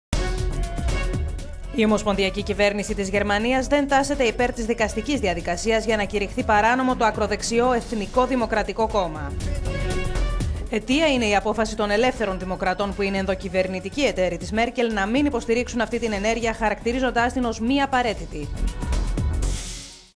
Γερμανία: Διαφωνία δικαστών κυβέρνησης για το...
Δεν τάσσεται υπέρ της δικαστικής διαδικασίας η ομοσπονδιακή κυβέρνηση της Γερμανίας, για να κηρυχθεί παράνομο το ακροδεξιό Εθνικό Δημοκρατικό Κόμμα.

Αιτία είναι η απόφαση των Ελεύθερων Δημοκρατών, που είναι κυβερνητικοί εταίροι της Άνγκελα Μέρκελ, να μην στηρίξουν αυτή την ενέργεια, χαρακτηρίζοντάς την ως μη απαραίτητη.